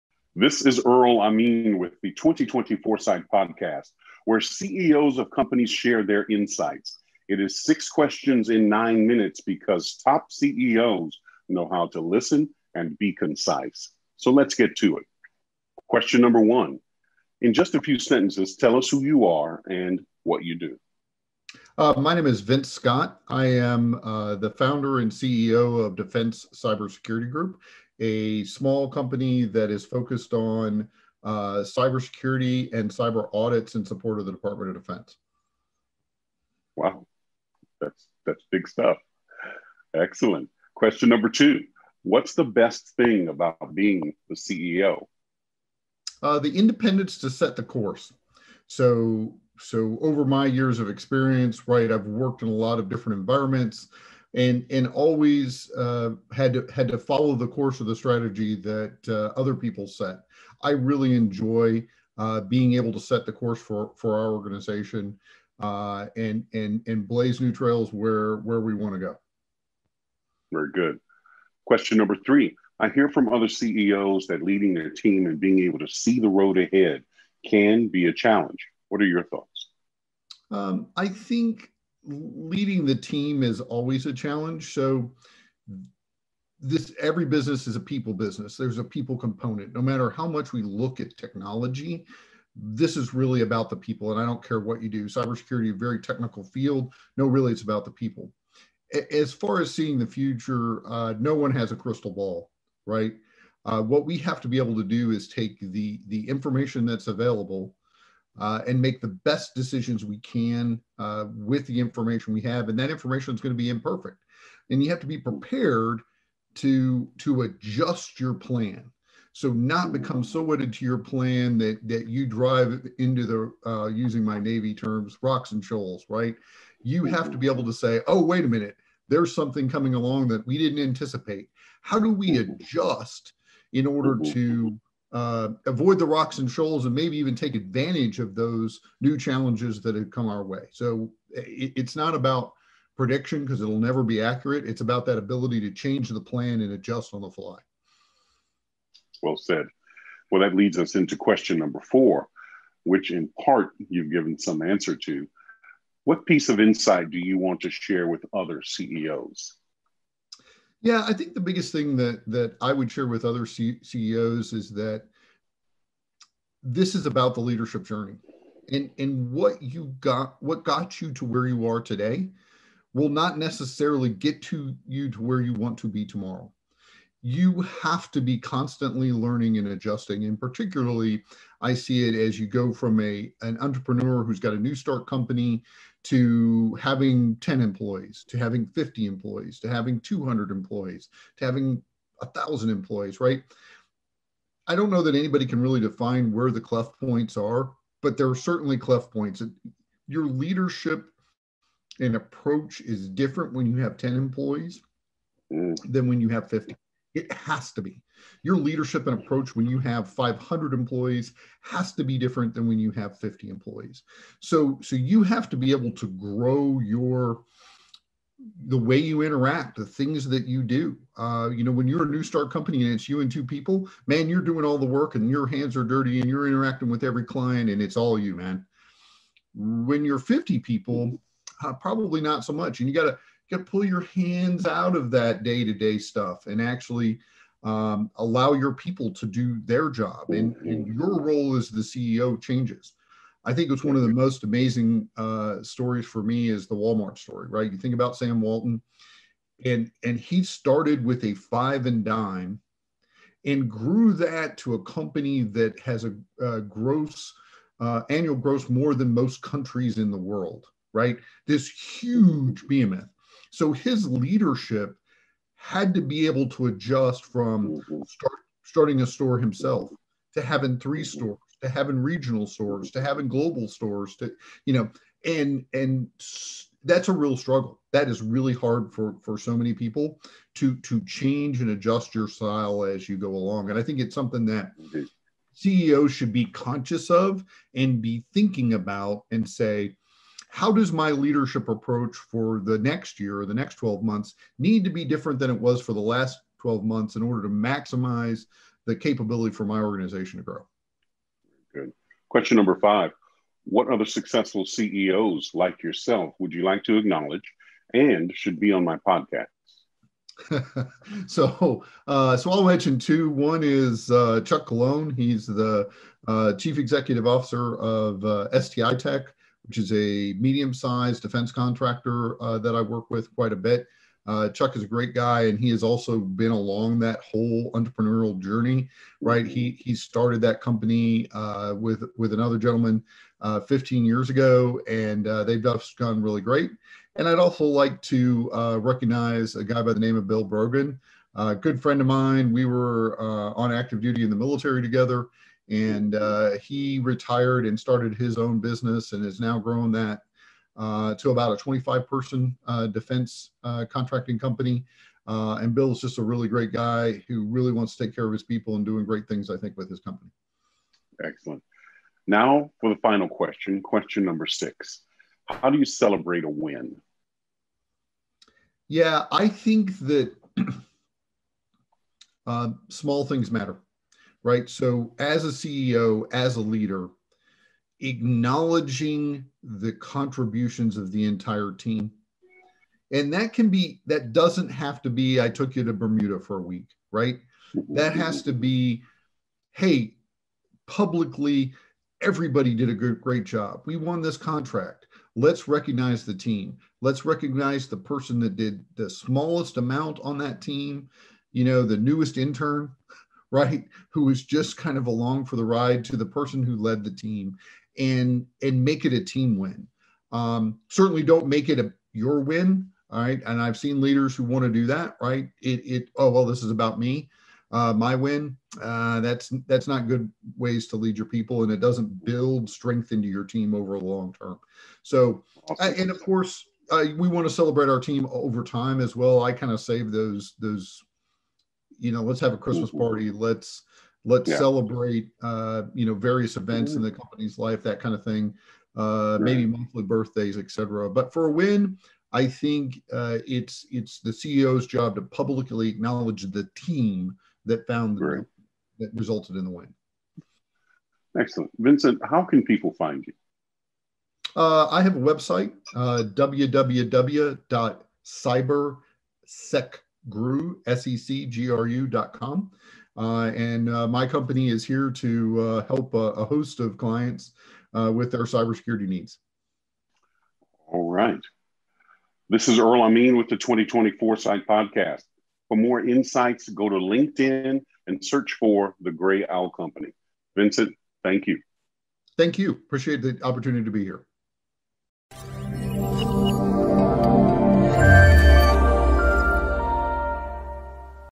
Ep 70: Vince Scott, Founder of Defense Cybersecurity Group.
How much cyber is enough in a small to medium sized business?